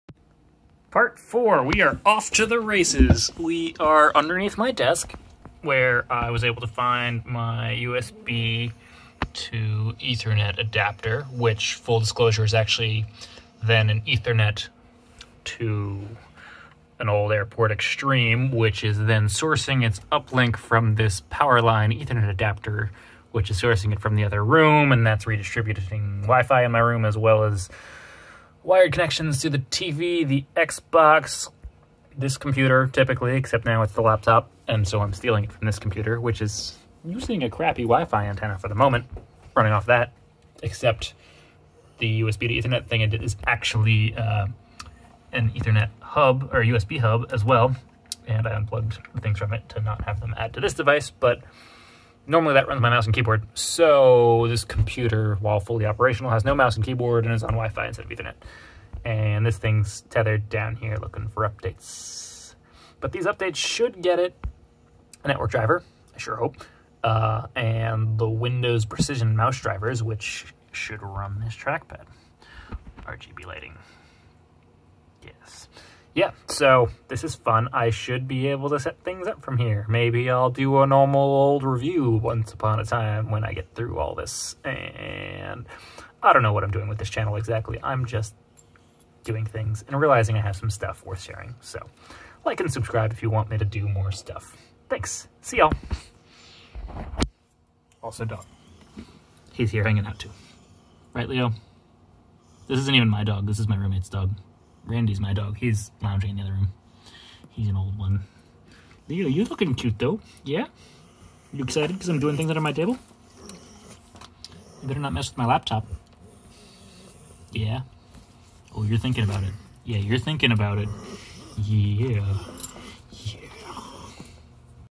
Razer Blade Stealth SSD Upgrade Part 4
Yeah one day I'll actually start editing... Probably using this fancy new computer. That'll actually be an interesting use of the gtx 1650 as well as this new SSD. I'll have to tear apart my old computer and use it's HDs for video file storage.

This is mostly me laying on the ground with a dog. Computer success though!